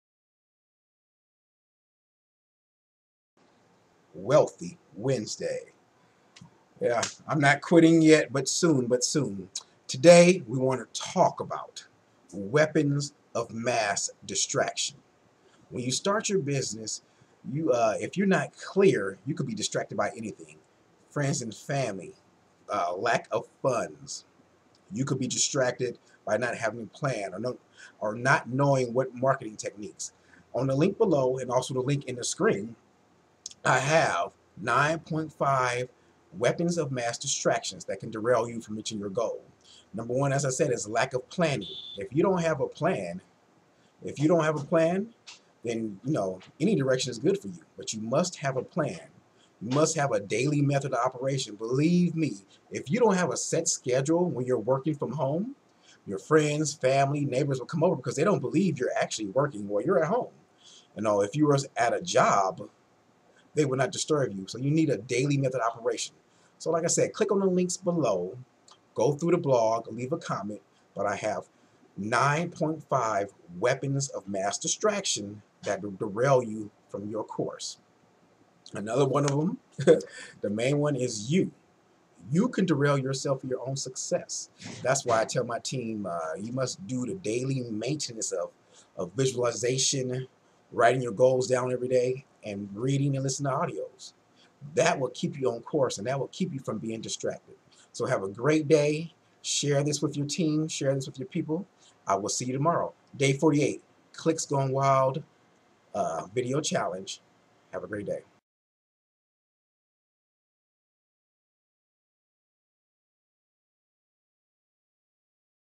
WMDs Weapons of Mass Distractions
Hey there entrepreneur! Did you just open up your doors? Are you a newbie to the online world? Or are you a new veteran that has been beat up and abused by the industry? It doesn’t matter what stage you are in your business. You are still vulnerable to Weapons of Mass Distraction (WMDs).

What are Weapons of Mass Distraction? They could be anything that take you away from achieving business success. The main culprits are your family, friends, shiny objects, not having a goal, failure to have a business plan, lack of capital, excuses, fear of competition, and the path of least resistance. They can destroy your business and having you applying for a greeter at Walmart.